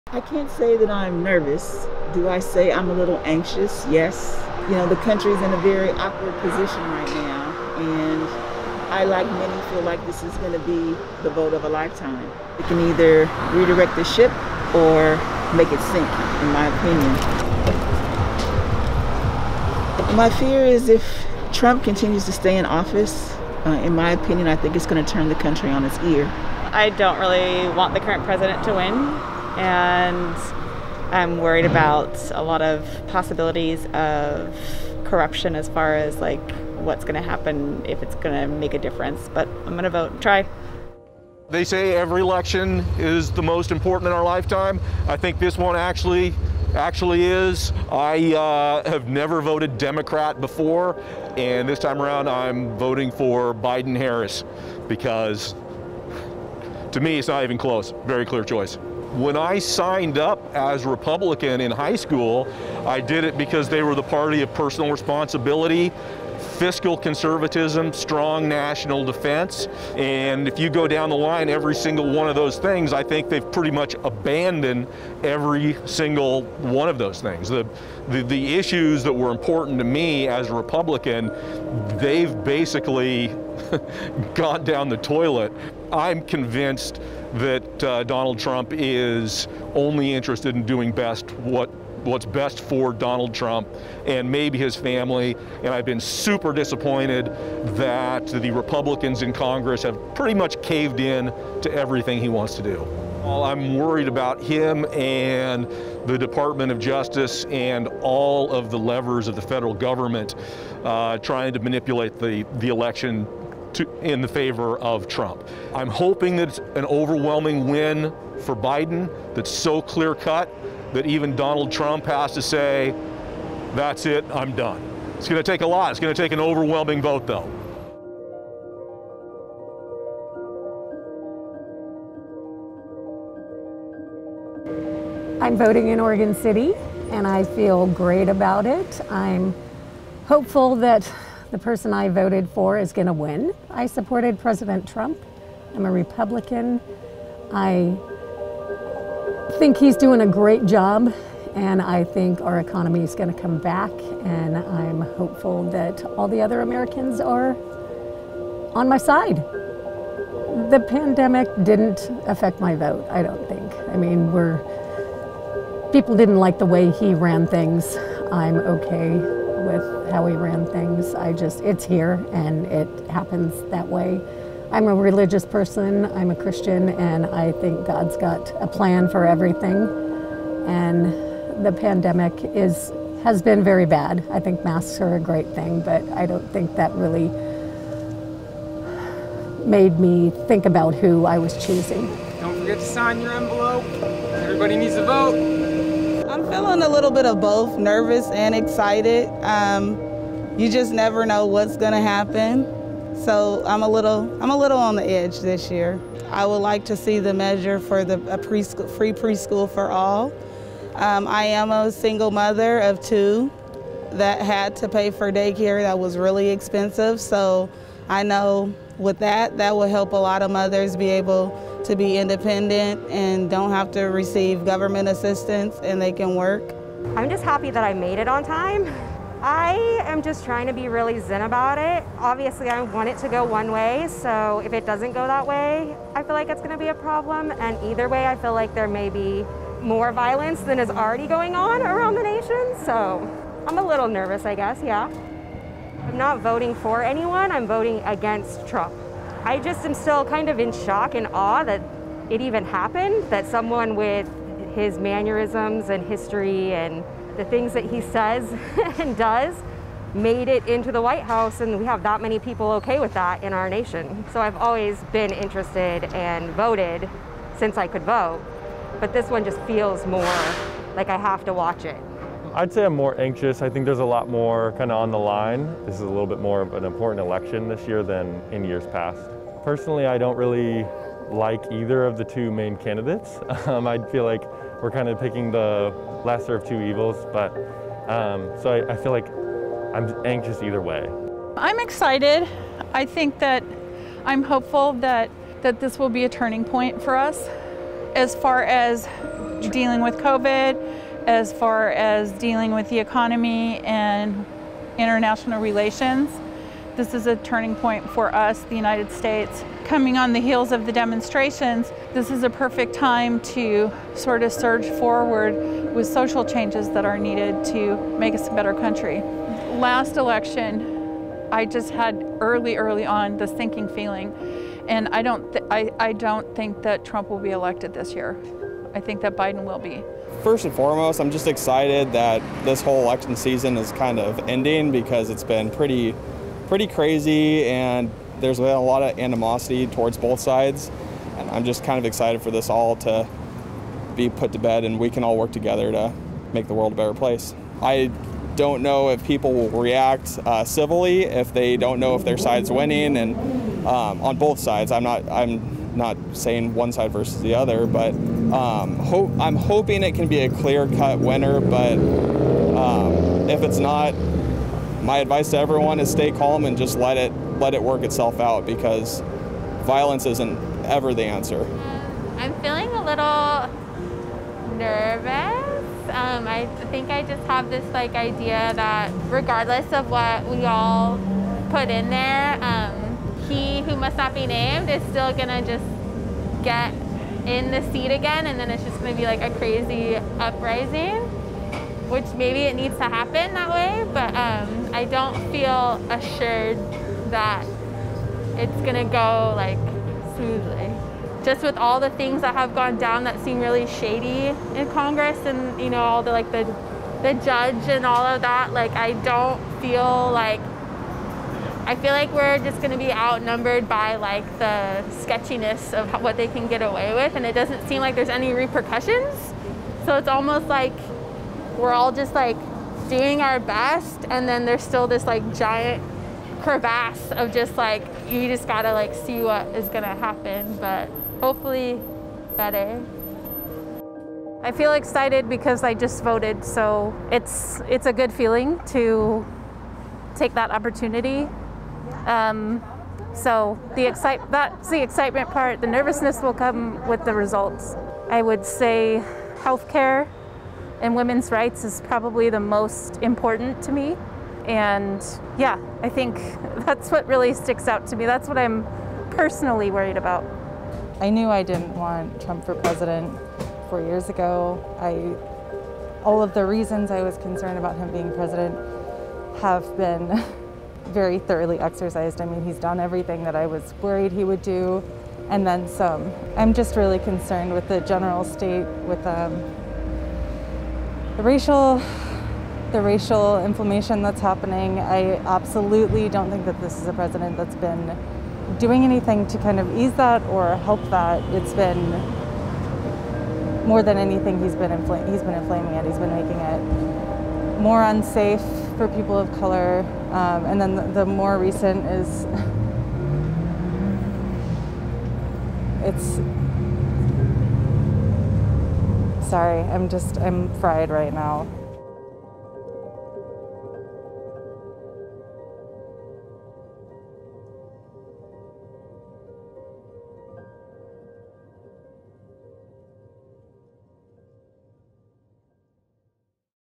Portlanders cast their votes in the 2020 election
Portland-area voters talk about the 2020 election as they cast their ballots.